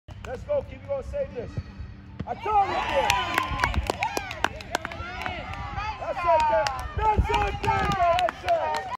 HCSA Penalty Save 12NOV22 ELLNER, BENJAMIN
Last game of the season for Hamden Soccer Association. Benjamin blocks a penalty shot inside the goal box